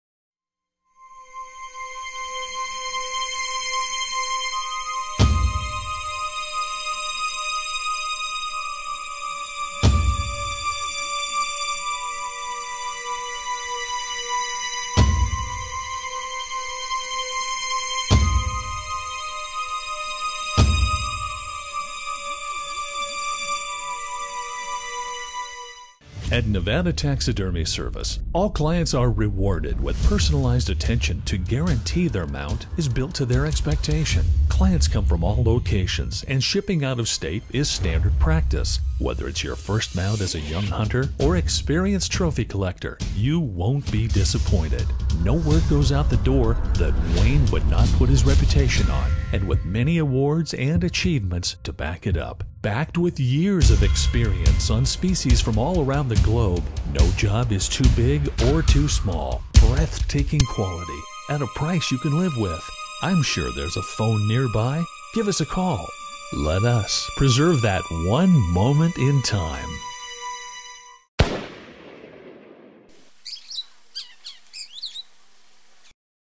Nevada Taxidermy segment.mp4
Wayne Comstock Nevada Taxidermy demo